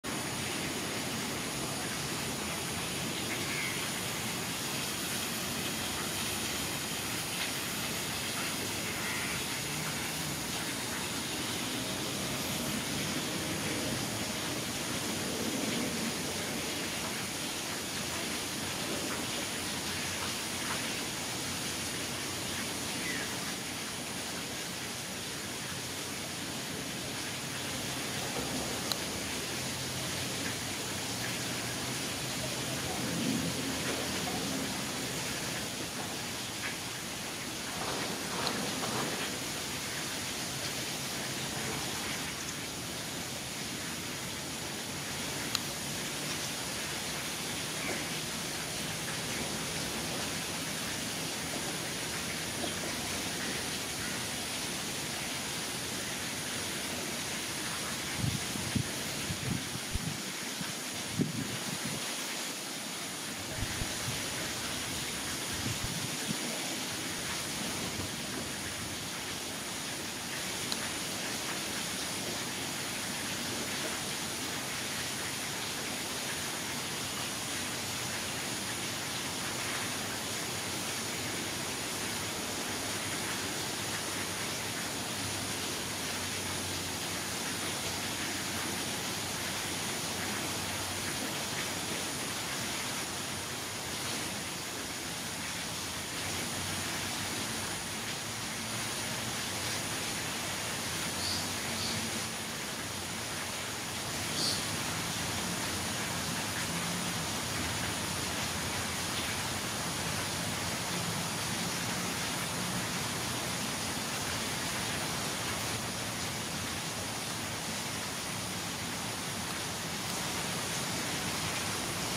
野生马来渔鸮/猫头鹰 _ Buffy Fish Owl Family In Dover Forest West _ 05Aug2023 Singapore 8.42am
PS.. nestling not sighted... both parents still highly alert.